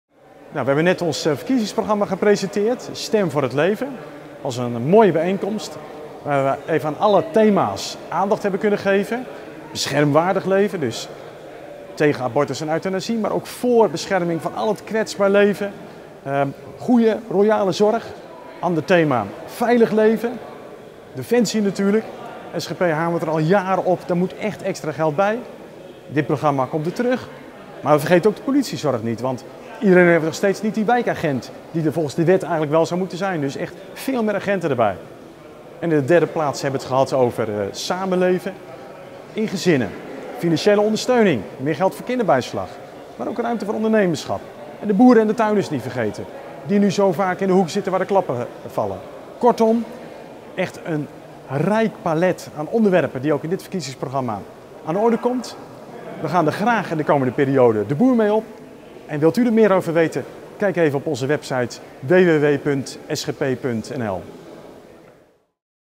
Presentatie Verkiezingsprogramma SGP
Op vrijdag 4 november 2016 presenteerde de SGP haar verkiezingsprogramma. Bescherming van kwetsbaar leven, steun voor gezinnen, meer geld voor Defensie en veiligheid, aandacht voor de landbouw en andere thema's: het staat er allemaal in.